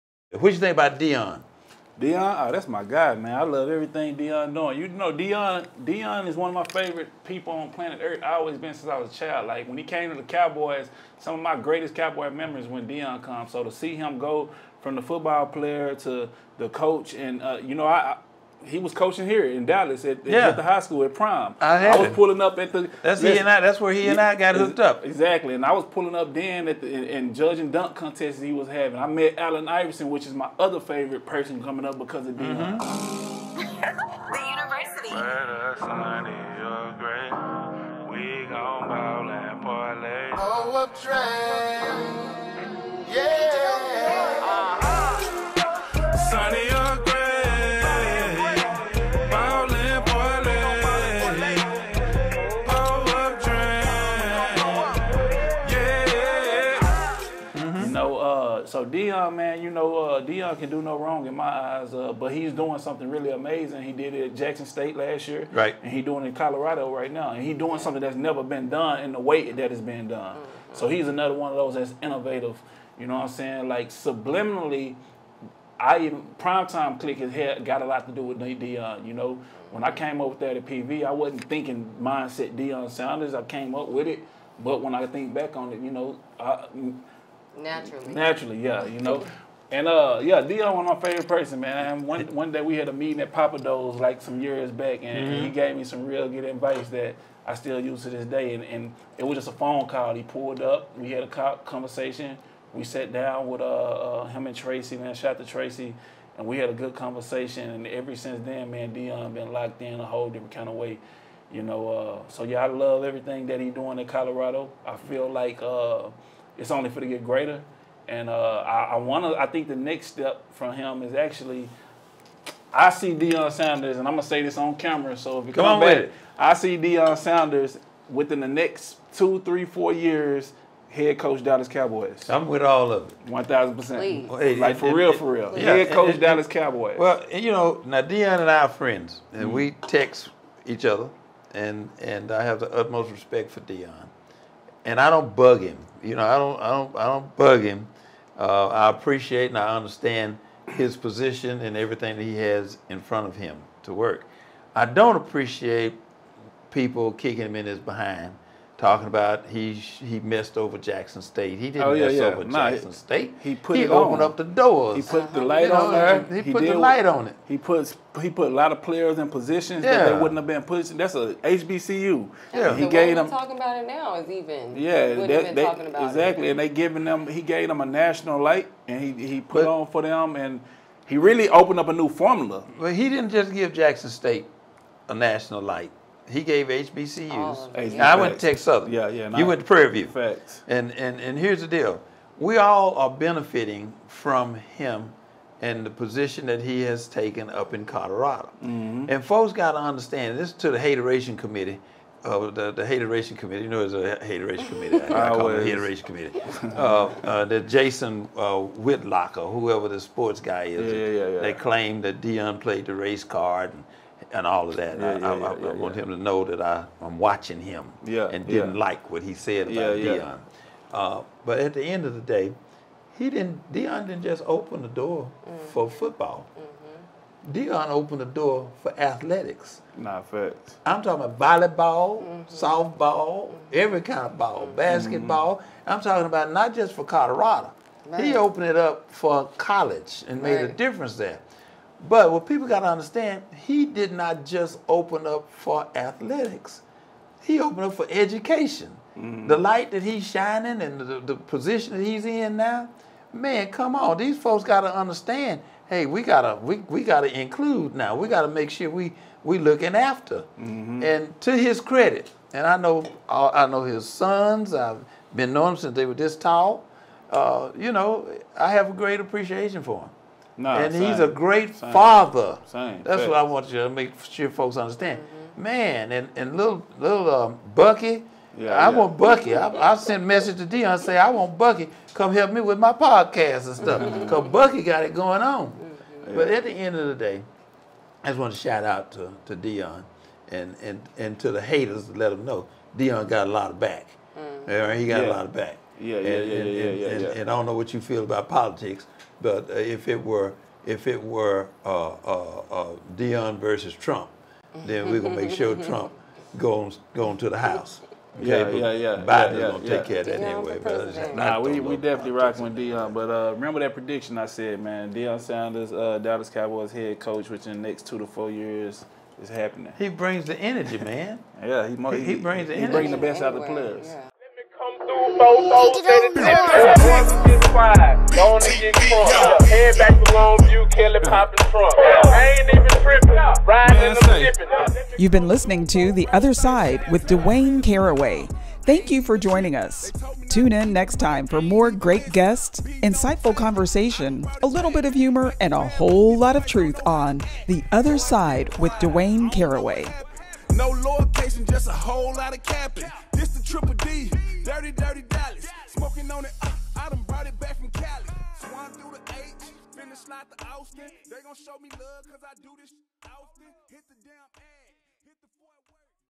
DORROUGH Talks about Deion Sanders
Listen to what Dorrough has to say about Deion Sanders on The Other Side Podcast with DC. You will be surprised by his influence .

@theothersidewithdc1 @Conservative Voices of Color @DorroughMusic